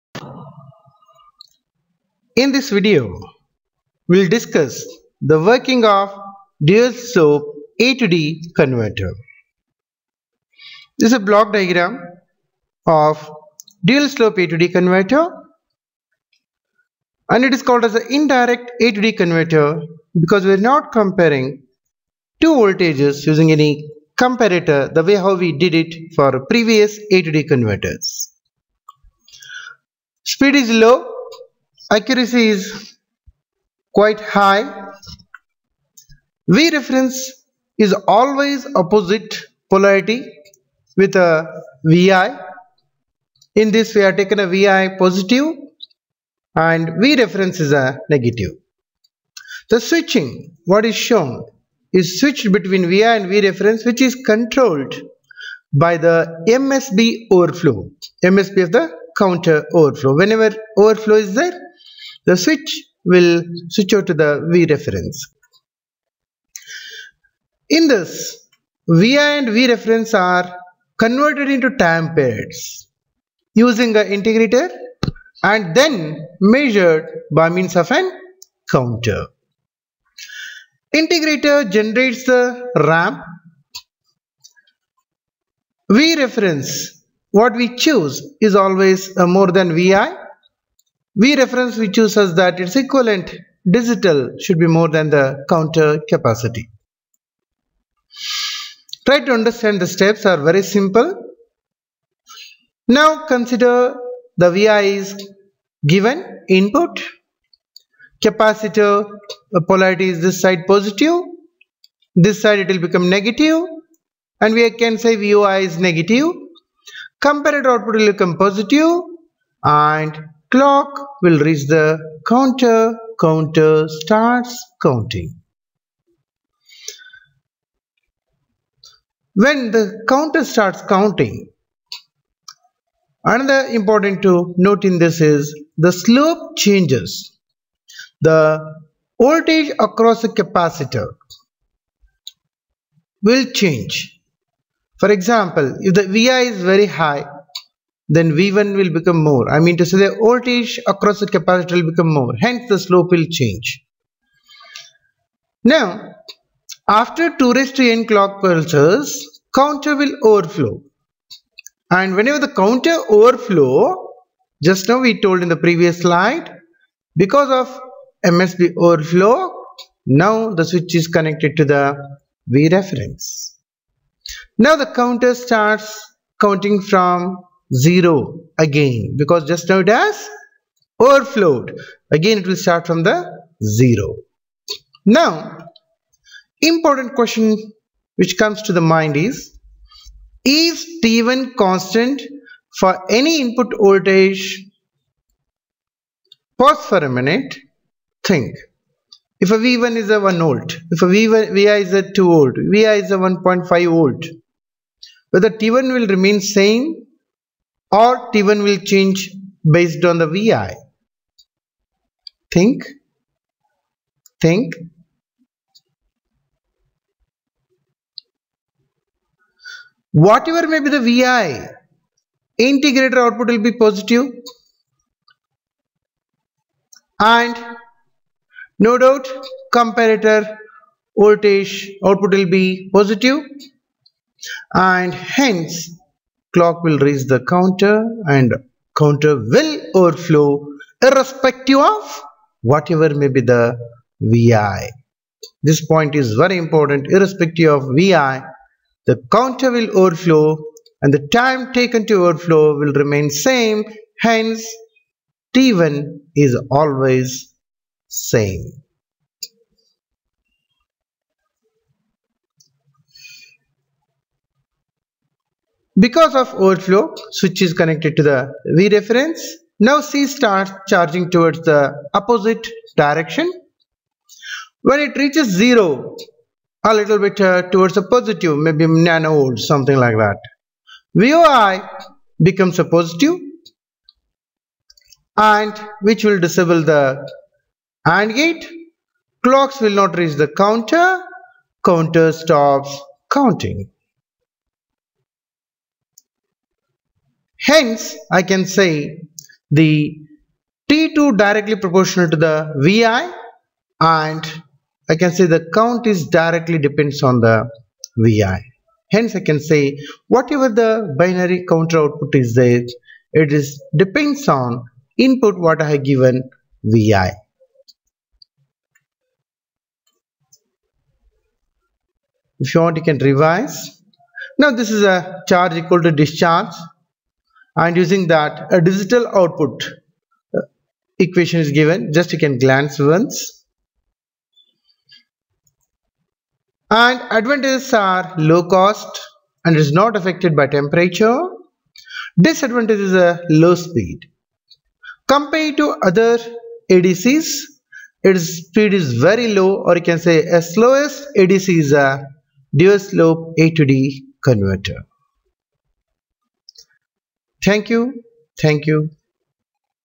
Dual slope ADC
Working of dual slope ADC is discussed in this video.